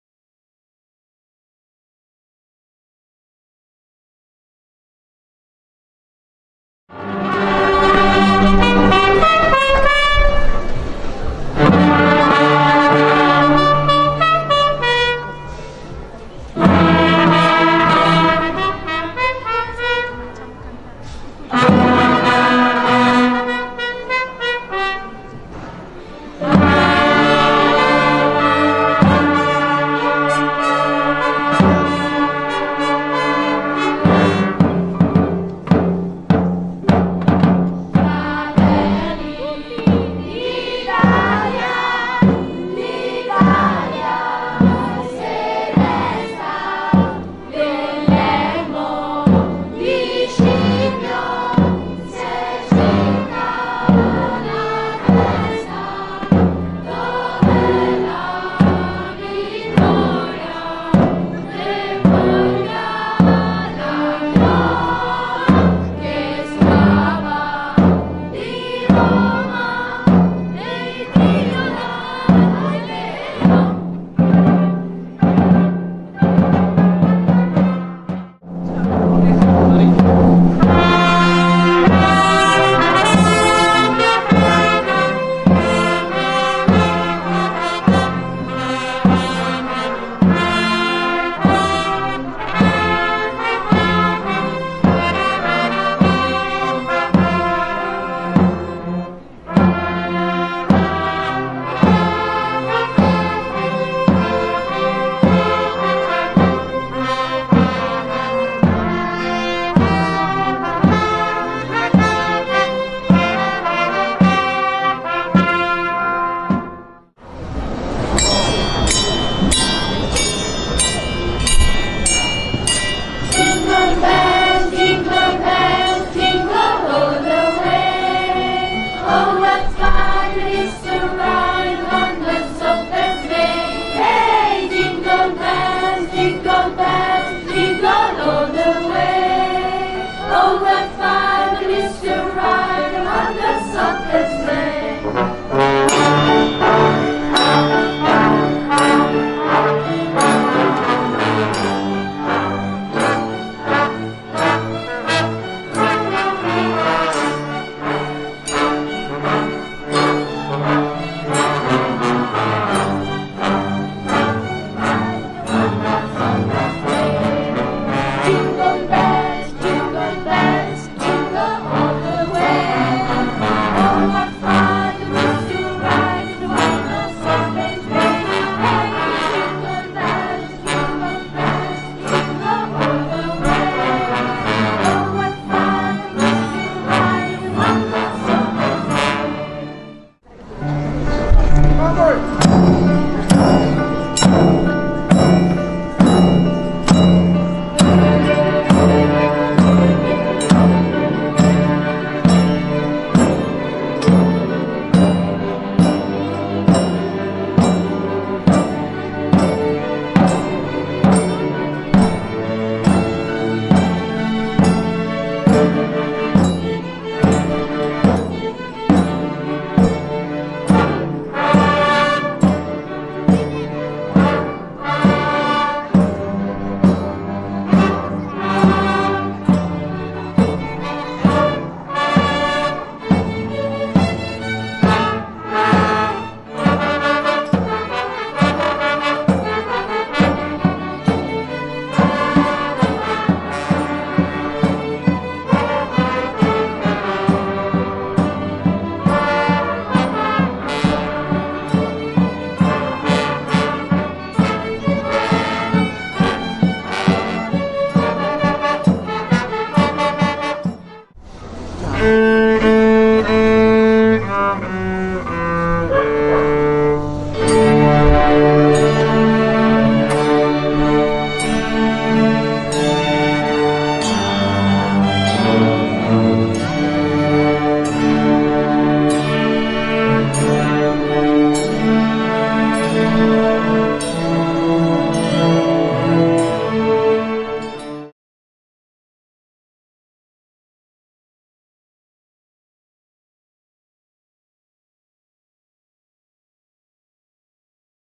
Flashmob 25 novembre 2022
Ecco alcuni momenti del Flashmob "Contro la violenza, musica e parole di speranza" tenuto dall'Orchestra Note di Classe dell'indirizzo musicale della Scuola Secondaria di Primo Grado Boccardo con il coro delle quinte delle scuole primarie dell'istituto.  25 novembre 2022, palazzo Dellepiane, Novi Ligure